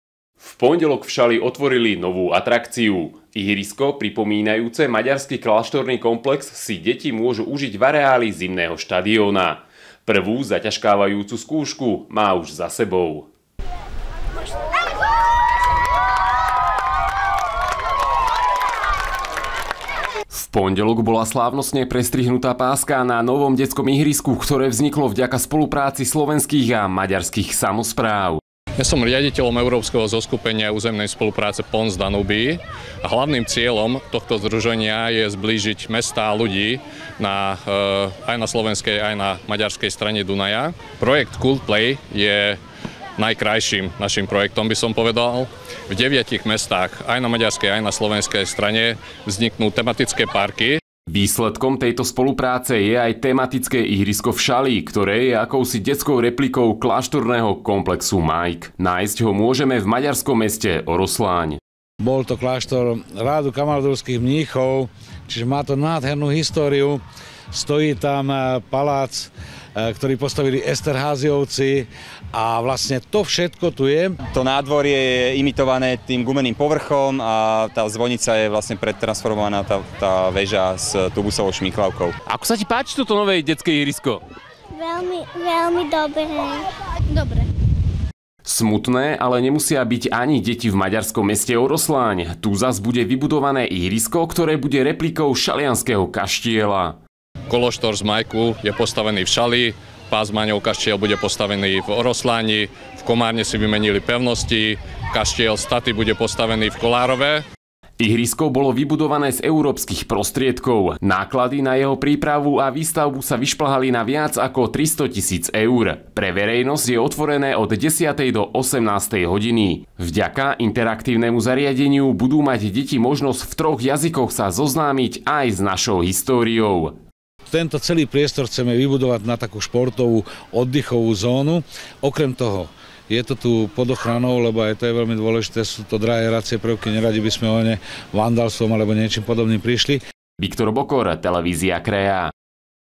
OTVORILI IHRISKO
V pondelok v Šali otvorili novú atrakciu. Ihrisko pripomínajúce maďarský kláštorný komplex si deti môžu užiť v areály zimného štadióna. Prvú zaťažkávajúcu skúšku má už za sebou.